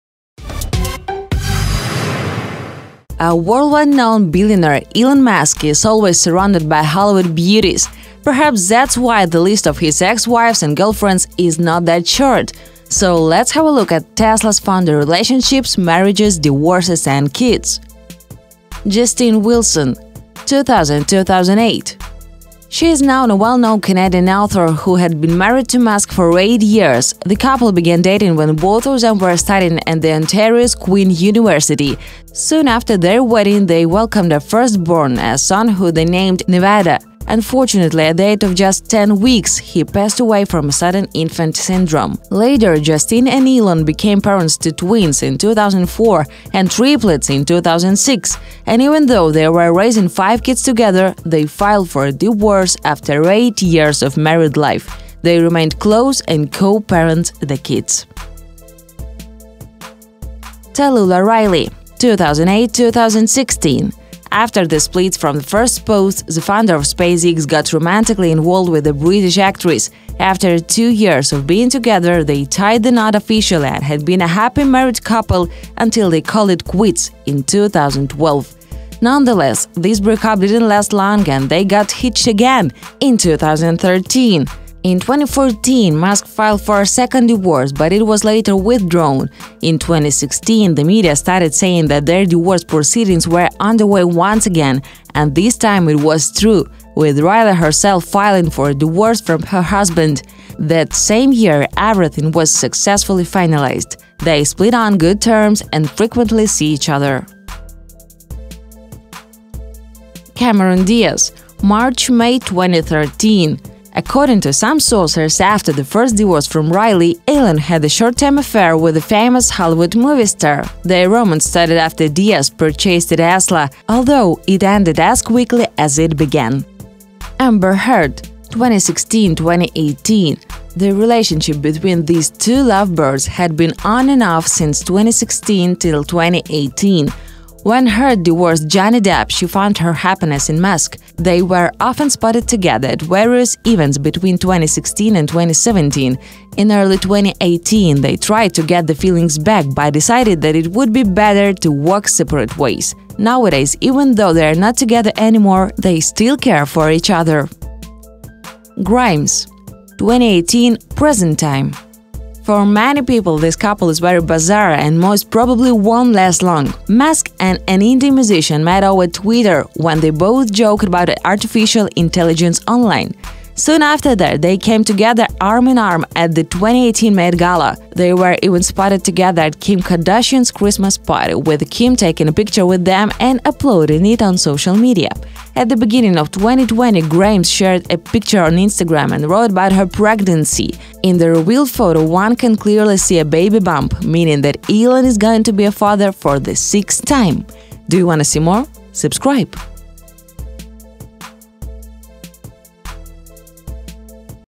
Elon Musk Girlfriends List: Dating History
A worldwide known billionaire Elon Musk is always surrounded by Hollywood beauties. Perhaps, that’s why the list of his ex-wives and girlfriends is not that short. So, let’s have a look at Tesla's founder relationships, marriages, divorces and kids.

Justine Wilson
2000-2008

She is now a well-known Canadian author, who had been married to Musk for 8 years. The couple began dating when both of them were studying at the Ontario’s Queen University. Soon after their wedding, they welcomed a firstborn, a son who they named Nevada. Unfortunately, at the age of just 10 weeks he passed away from a sudden infant syndrome. Later, Justine and Elon became parents to twins in 2004 and triplets in 2006. And even though they were raising five kids together, they filed for a divorce after eight years of married life. They remained close and co-parent the kids.

Talulah Riley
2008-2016

After the spit from the first spouse, the founder of SpaceX got romantically involved with a British actress. After two years of being together, they tied the knot officially and had been a happy married couple until they called it quits in 2012. Nonetheless, this breakup didn’t last long and they got hitched again in 2013. In 2014 Musk filed for a second divorce but it was later withdrawn. In 2016, the media started saying that their divorce proceedings were underway once again. And this time it was true, with Riley herself filing for a divorce from her husband. That same year everything was successfully finalized. They split on good terms and frequently see each other.

Cameron Diaz
March-May 2013

According to some sources, after the first divorce from Riley, Elon had a short-term affair with a famous Hollywood movie star. Their romance started after Diaz purchased a Tesla. Although, it ended as quickly as it began.

Amber Heard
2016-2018

The relationship between these two love birds had been on and off since 2016 till 2018. When Heard divorced Johnny Depp, she found her happiness in Musk. They were often spotted together at various events between 2016 and 2017. In early 2018 they tried to get the feelings back but decided that it would be better to walk separate ways. Nowadays, even though they are not together anymore, they still care for each other.

Grimes
2018-present time

For many people, this couple is very bizarre and most probably won’t last long. Musk and an indie musician met over Twitter when they both joked about artificial intelligence online. Soon after that, they came together arm-in-arm at the 2018 Met Gala. They were even spotted together at Kim Kardashian’s Christmas party with Kim taking a picture with them and uploading it on social media. At the beginning of 2020 Grimes shared a picture on Instagram and wrote about her pregnancy. In the revealed photo one can clearly see a baby bump, meaning that Elon is going to be a father for the sixth time.